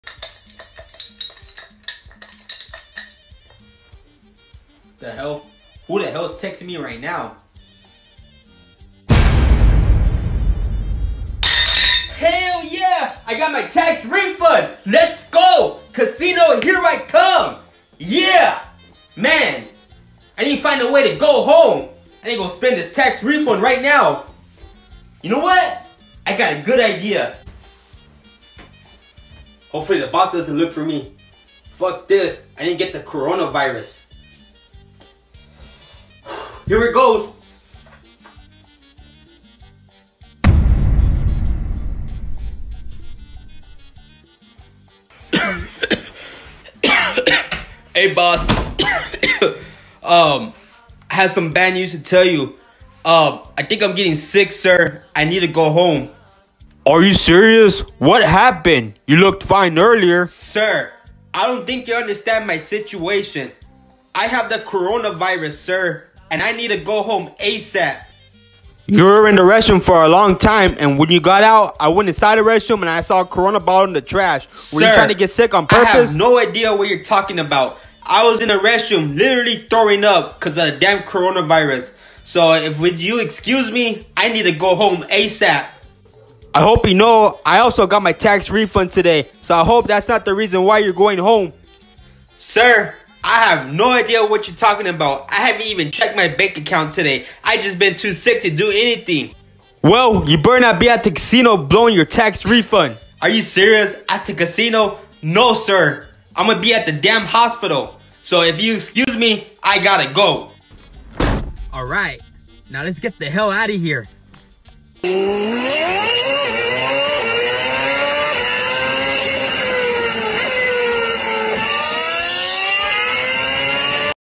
Corona Virus
When you gotta do whatever it takes to go home when your tax refund hits your direct deposit while at work lol.